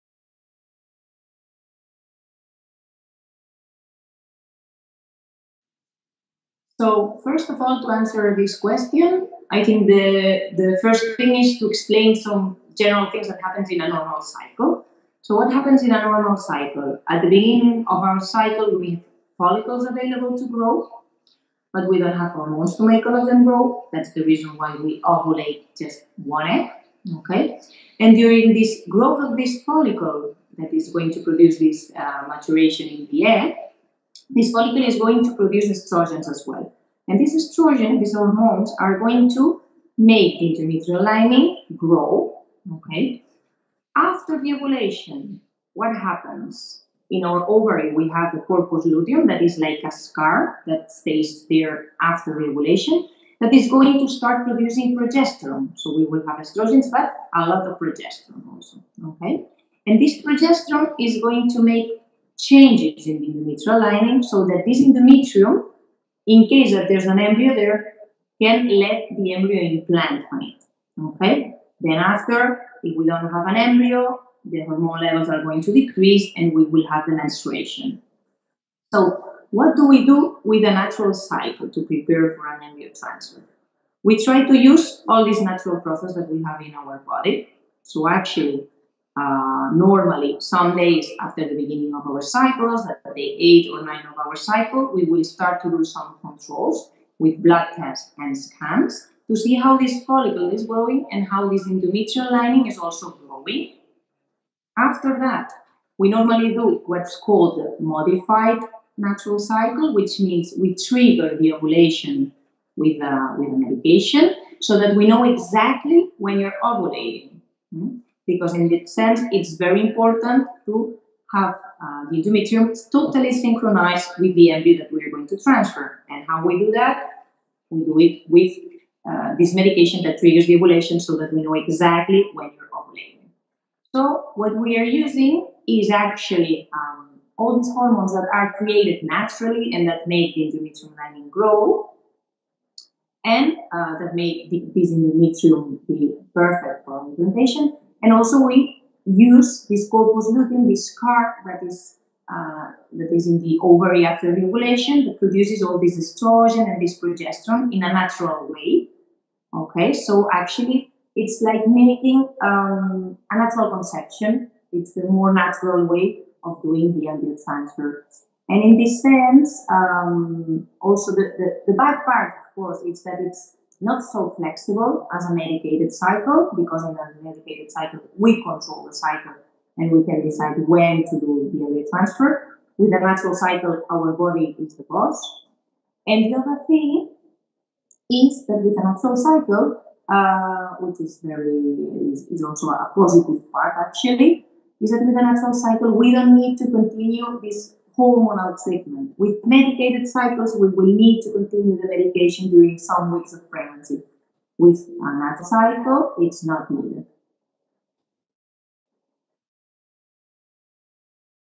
What is a natural FET cycle?
Dr. Anna Voskuilen, Reproclinic, is talking about natural frozen embryo transfer.